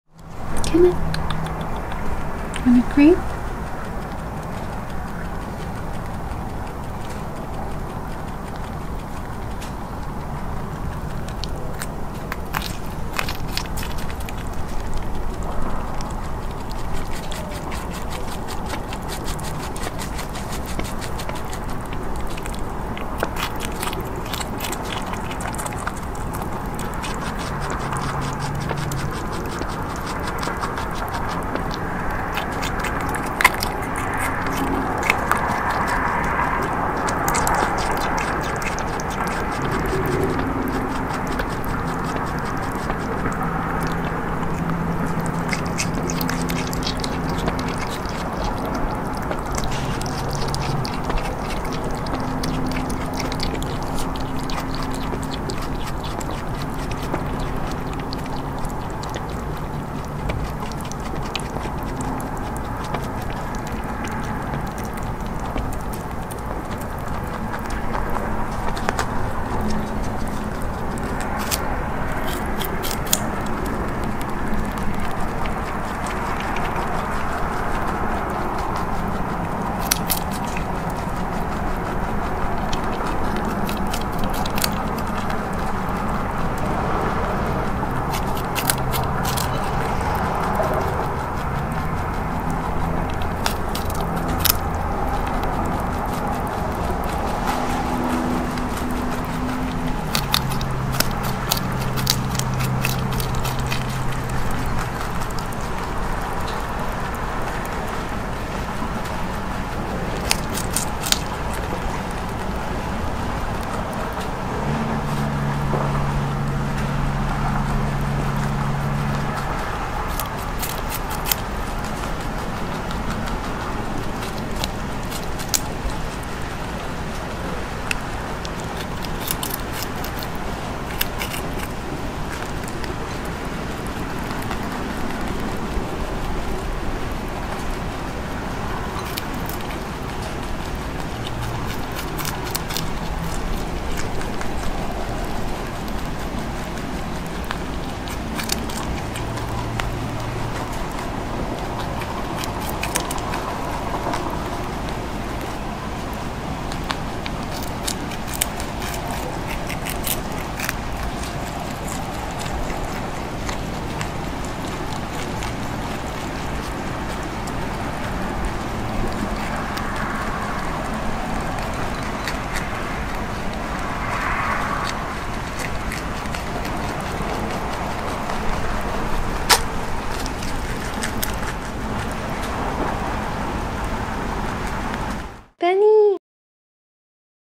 ASMR Wild mother bunny deserves pampering with various treats: grape, carrot, oats
A wild mother rabbit named Madame Bunny deserves to be pampered. Watch how this wild bunny munches on her various favorite treats: grapes, carrot, and oats. This cottontail lost her entire litter (baby bunnies) in the 2021 Texas winter storm. On the night her human friends buried the babies for her, they offered this wild bunny love, petting, and yummy snacks to comfort her.
Main character: wild bunny Madame Bunny

野兔媽媽邦妮的人類朋友用她最愛的點心寵愛她，因為她在德州暴風雪中失去了所有的野兔寶寶。人類在幫忙埋葬完野兔寶寶的當晚，給了野兔邦妮葡萄、紅蘿蔔和燕麥片，以及充滿愛的摸摸和秀秀，盼能好好安慰她。來看看野兔媽媽大聲享受點心的過程吧。
品種：棉尾兔　主角：野兔邦妮
拍攝日 02192021